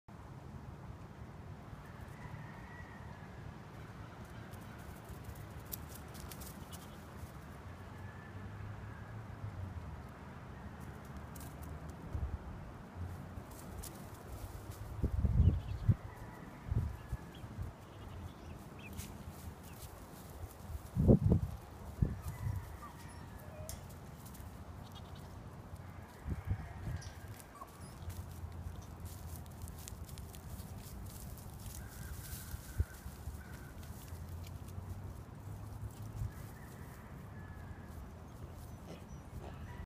Baby Goats. Oreo and Bambi.
1 week old Nigerian Dwarf baby goats.